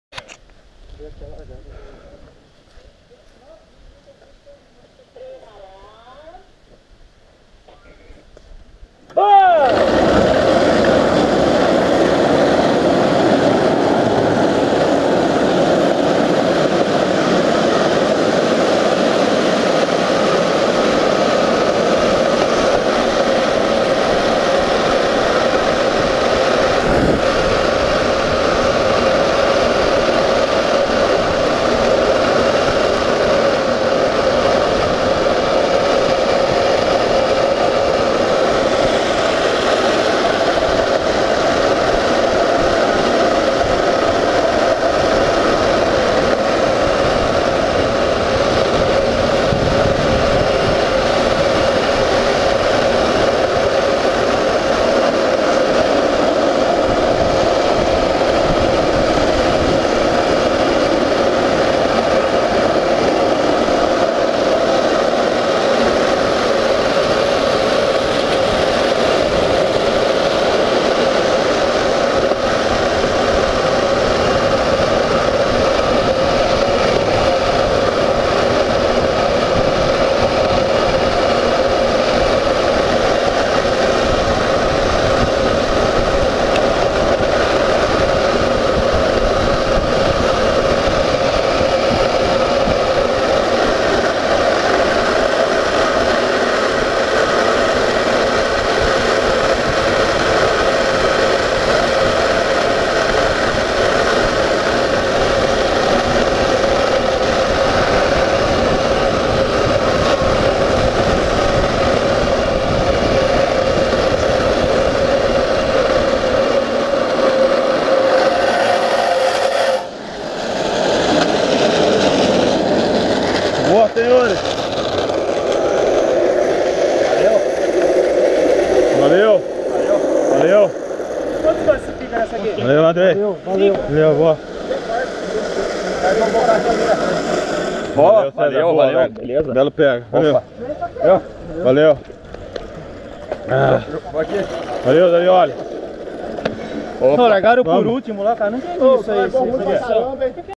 2 GP Chico Bento de Rolimã - Semifinais
Grande Prêmio de Carrinhos de Rolimã Chico Bento, na cidade de São Bento do Sapucaí, aos pés da Pedra do Baú